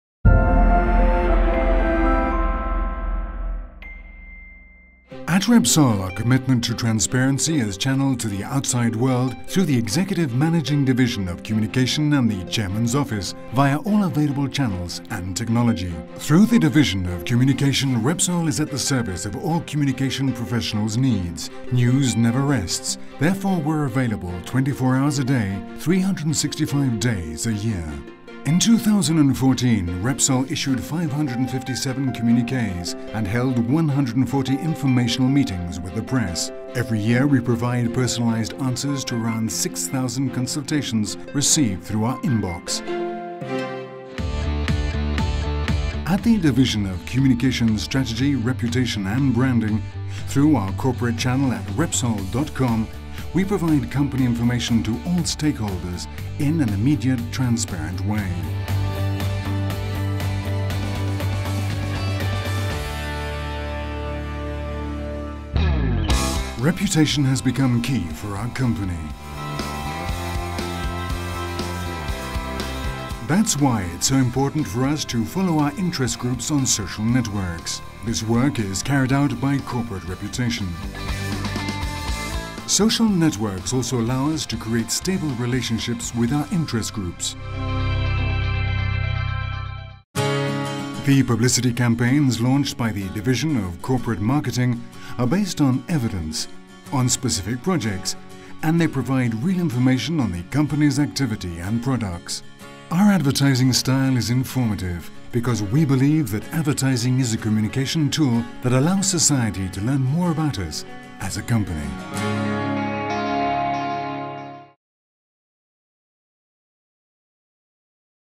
Repsol: Our communication is carried out with maximum transparency
Because news can happen at any time, at Repsol we are always ready to provide immediate information to our stakeholders to help them better understand our business.